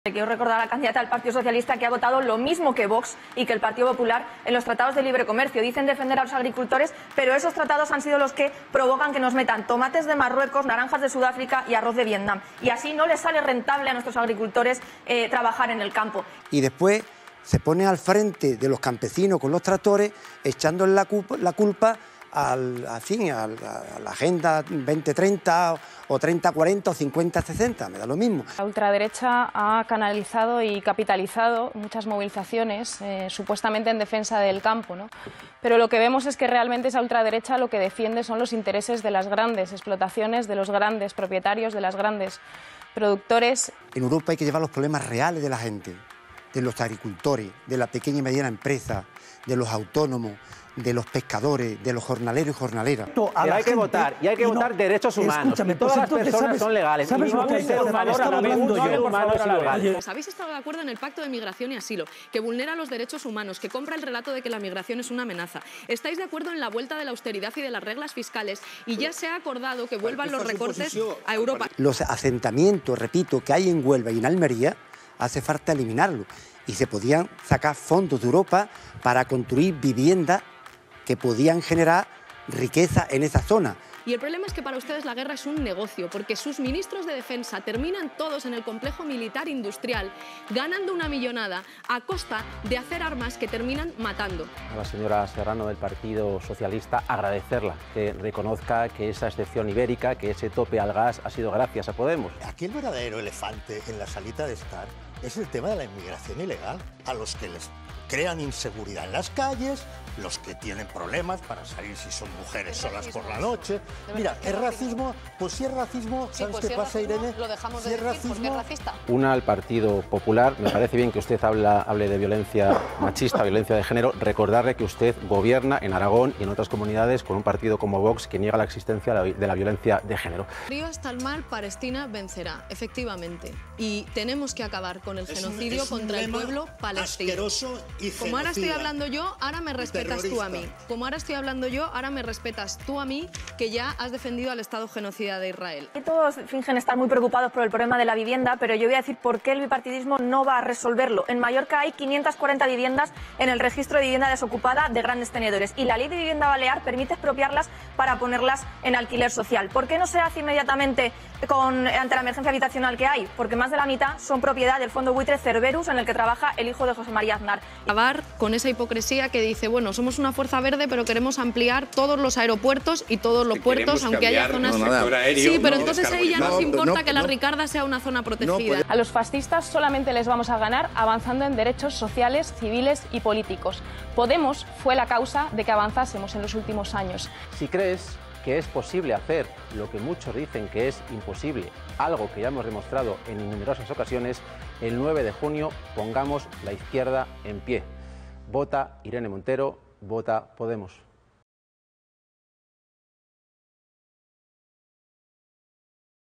Los mejores candidatos a las EUROPEAS son de PODEMOS 💜✊
Te traemos una recopilación de los mejores momentos de los candidatos de Podemos a las elecciones europeas en los debates de estos días en televisión.